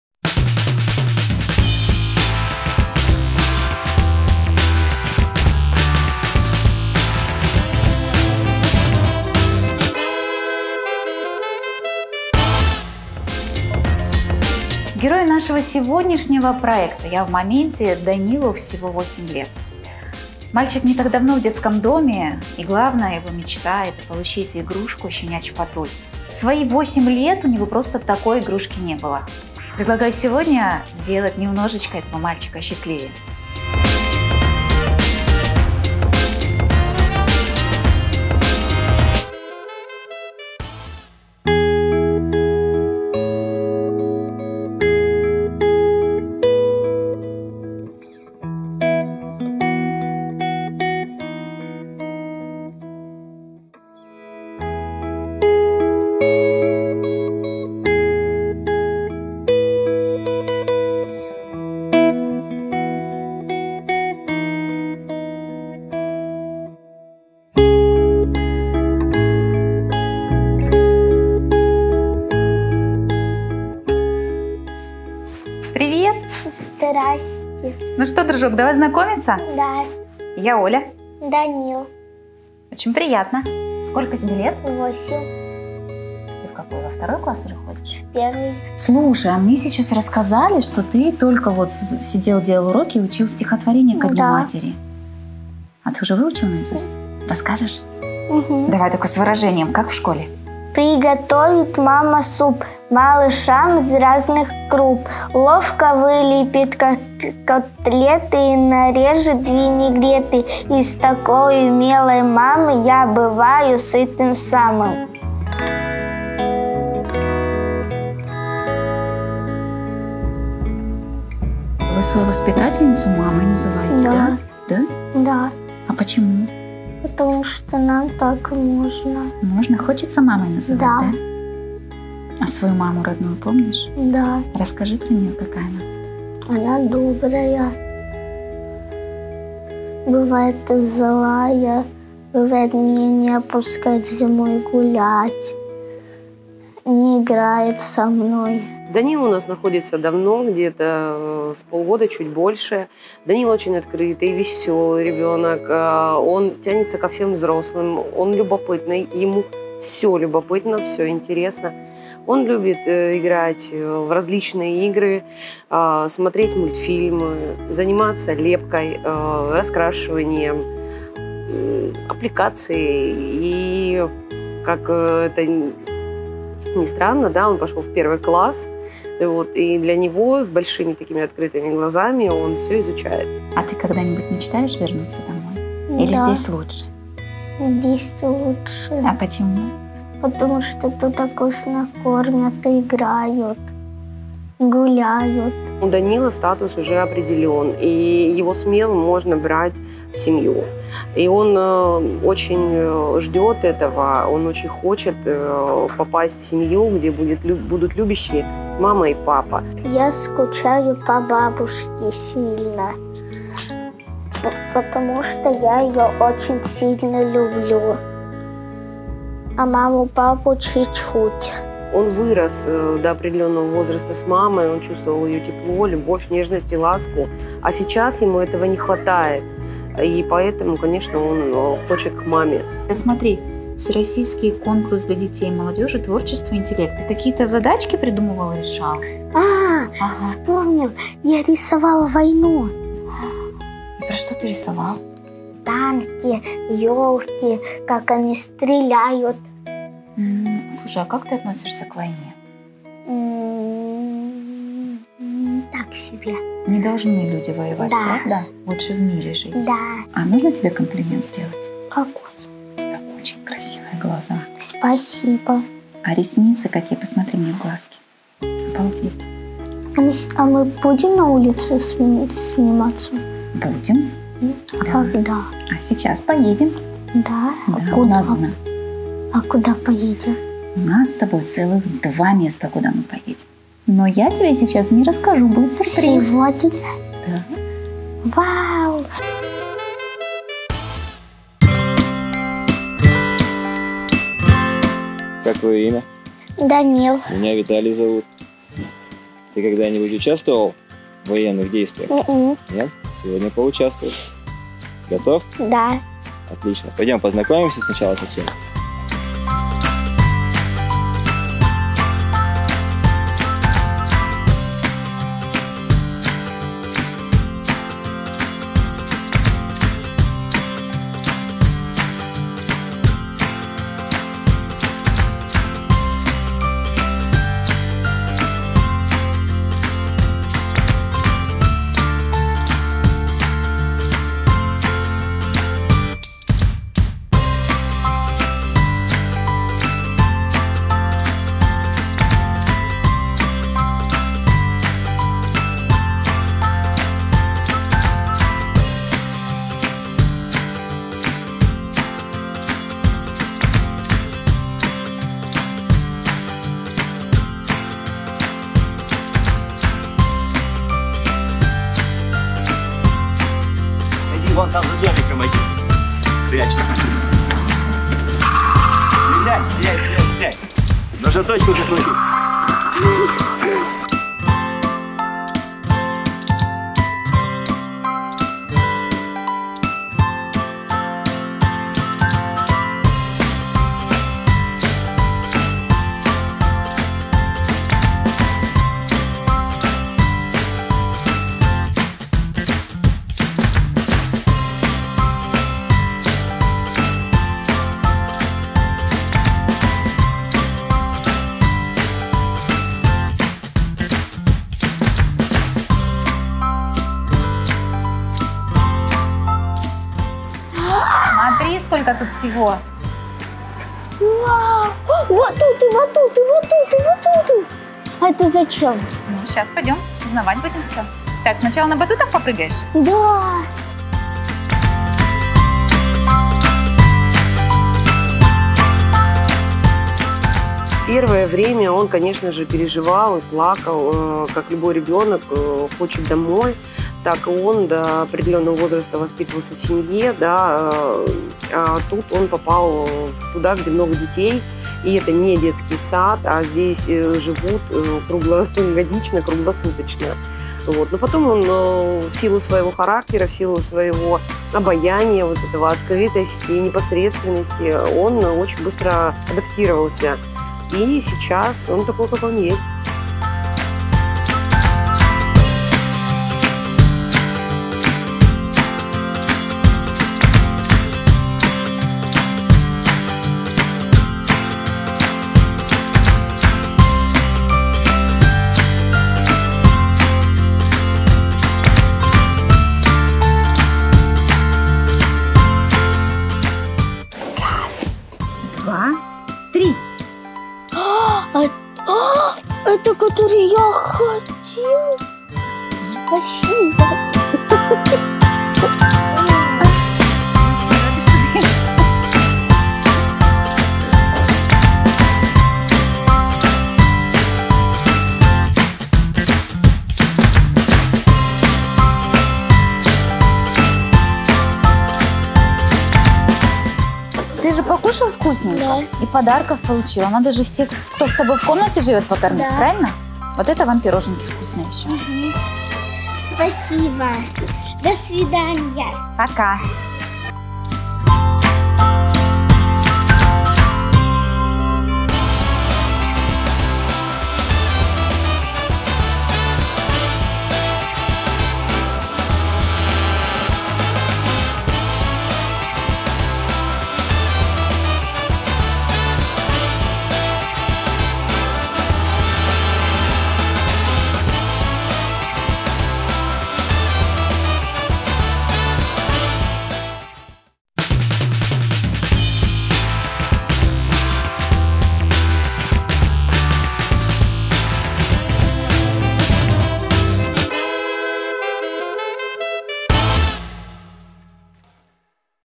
Я в моменте №5 08.12.2021
Мы начинаем делиться с вами улыбками детей! Смотрите пятую серию нового проекта.✔️ «Я в моменте» - социальный проект, созданный @telemiks для помощи детям, которые оказались в трудной жизненной ситуации. Наша цель - поддержать детей, видеть их улыбку и радость.

▫️ партнёр спортивно-развлекательный центр "Глобал плэй" @global_play_uss

▫️ Выражаем благодарность отделу опеки и попечительства и спортивному клубу "Патриот"